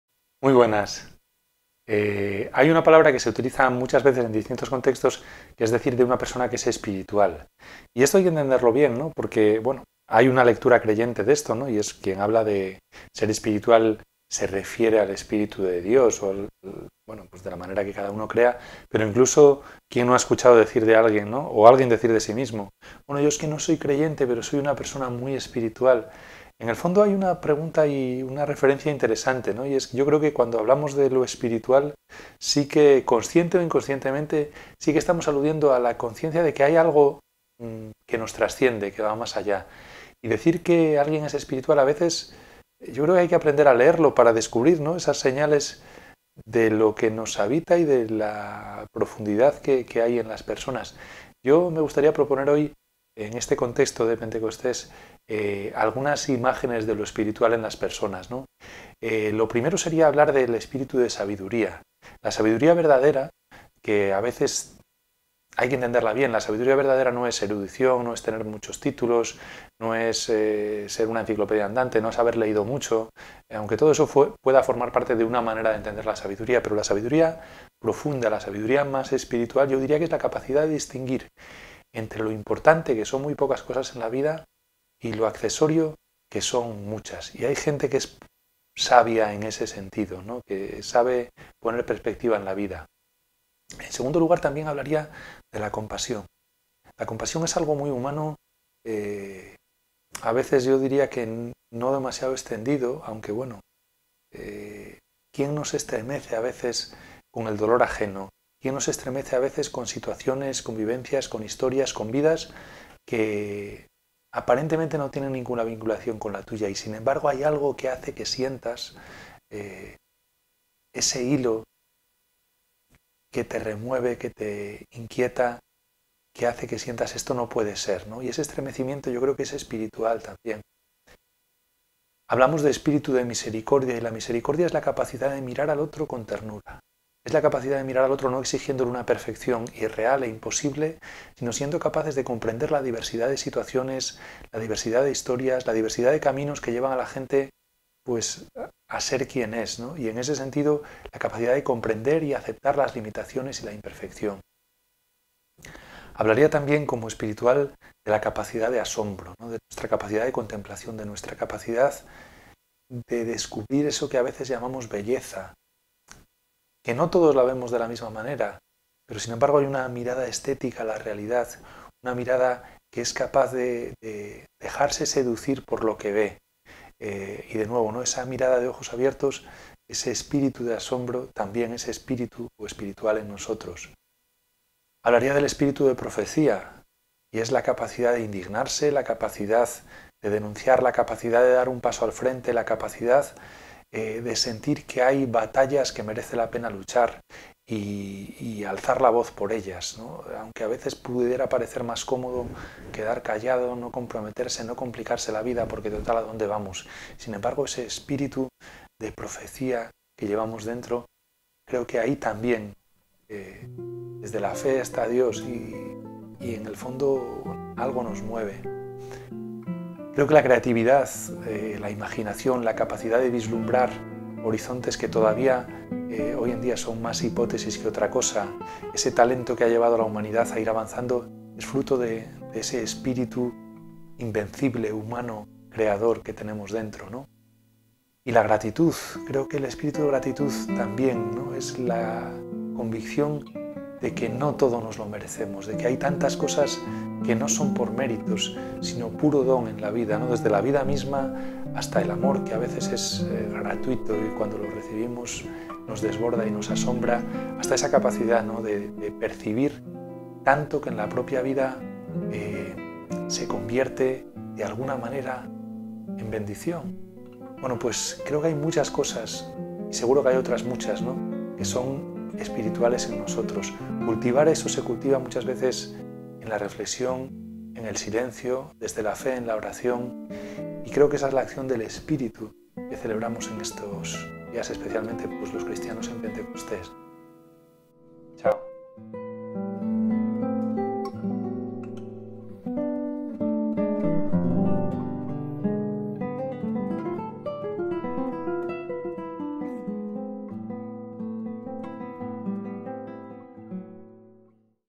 Vivir por dentro
¿Qué queremos decir de alguien cuando decimos que es espiritual? ¿Qué es ese "espíritu"? ¿Y qué efectos tiene en las personas?